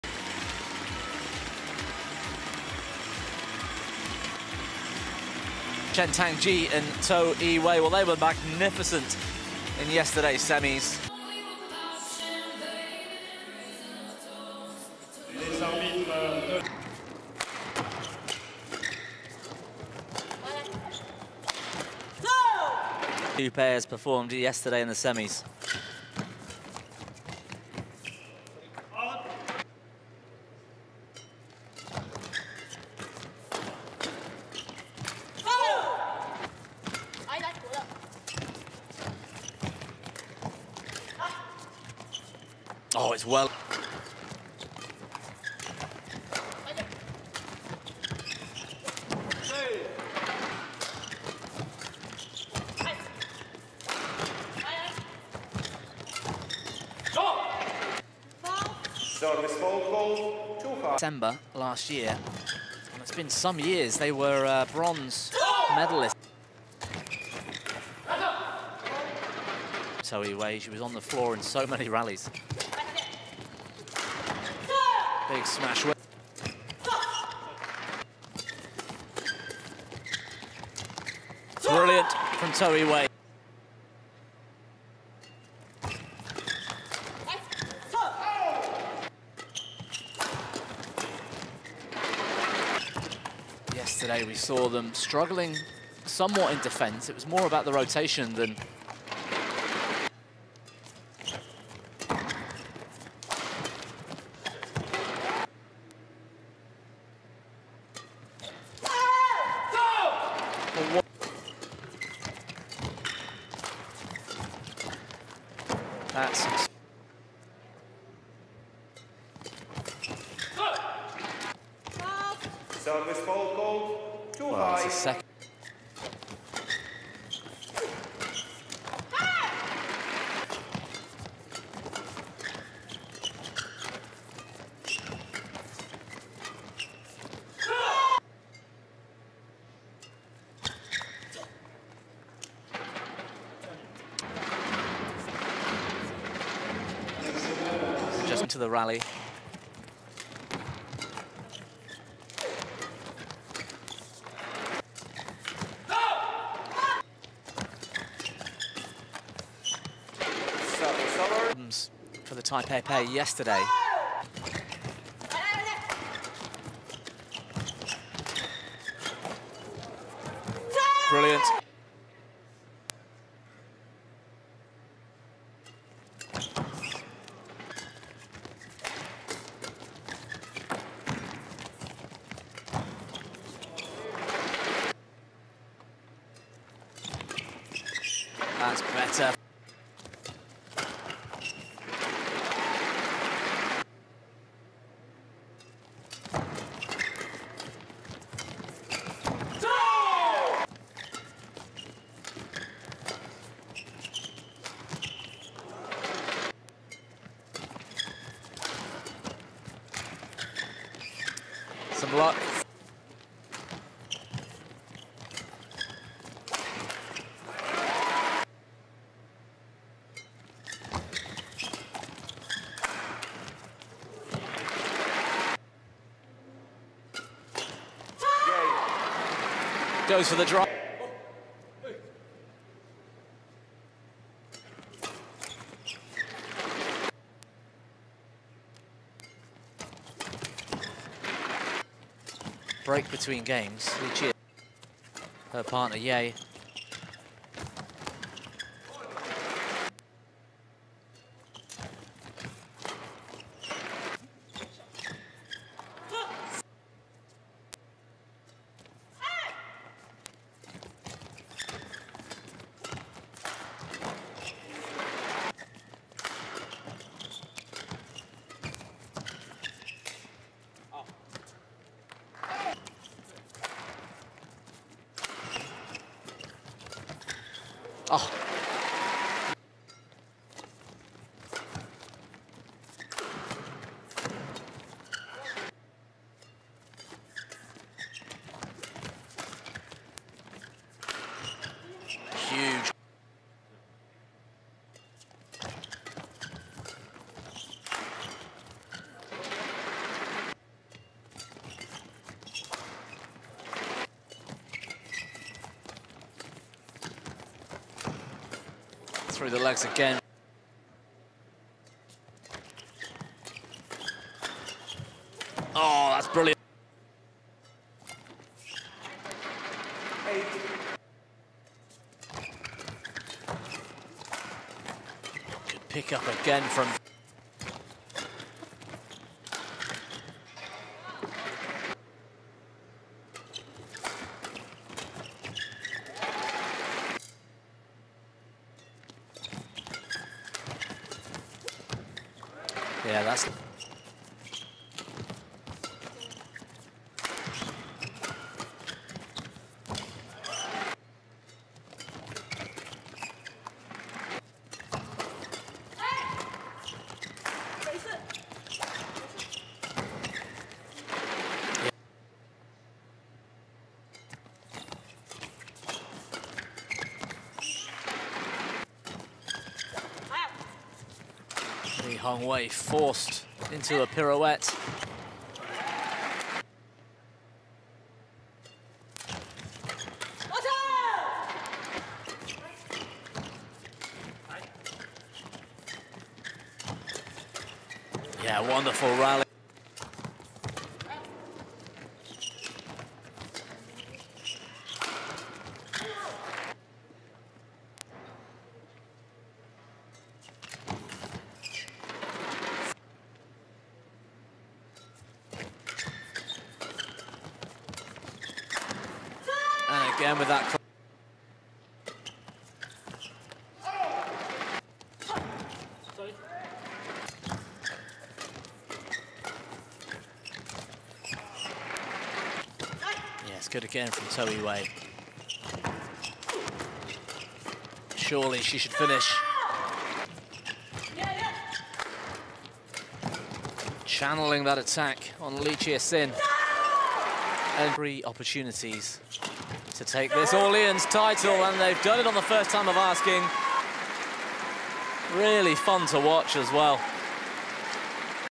[FINAL]🇲🇾Chen Tang Jie-Toh Ee Wei MUNCUL Juara Orleans Masters👏🤩‼️orleansmasters2023
‼️Pencapaian TERBAIK🔥‼️

🇲🇾Chen Tang Jie-Toh Ee Wei JUARA ORLEANSMASTERS2023